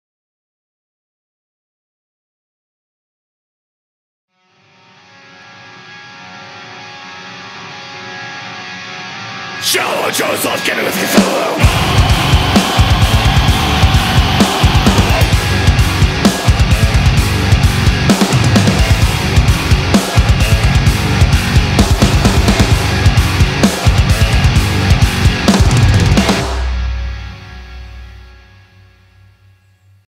New Outro By: Shallow Truths
Behold! my new permanent outro for my channel. My good friend Jacob Myers from the band Shallow Truths recorded this for me. I am beyond happy that I finally found an artist that was humble enough to take this request on for me, and make a masterpiece.

The outro for this channel was done by the band Shallow Truths: Vocalist - Jacob Myers